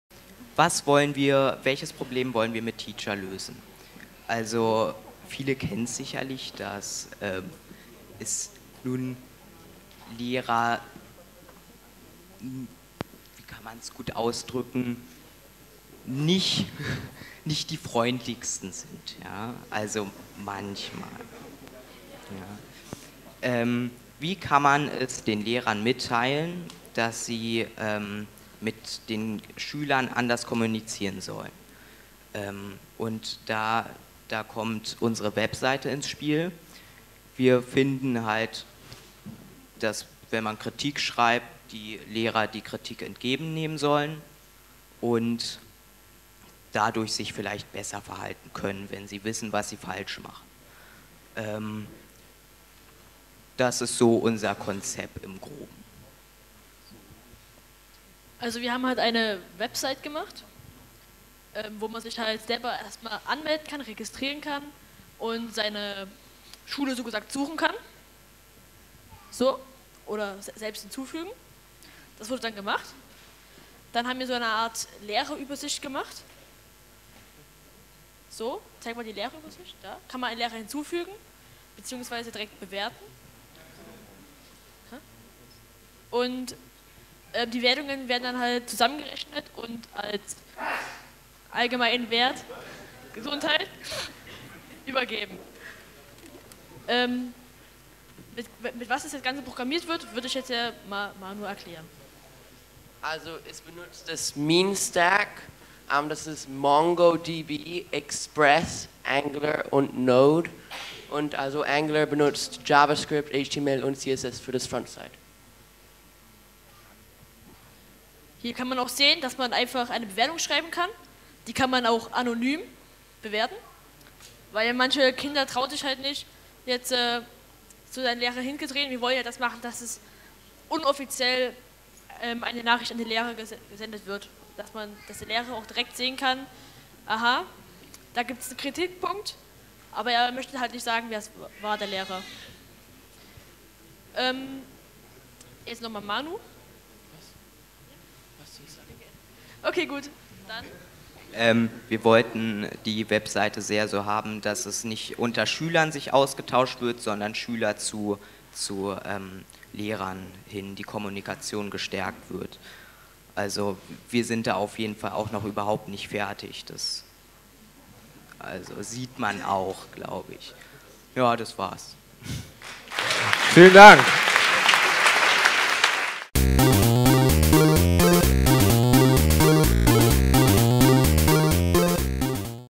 Teecher | Jugend hackt Berlin 2015
In der Regel bewerten Leher*innen ihre Schüler*innen. Das Projekt „Teecher“ dreht den Spieß um und gibt Schüler*innen die Möglichkeit ihre Lehrer zu bewerten und Feedback zu geben. Von Manu, Noah, Robin, Merlin und Jonnas.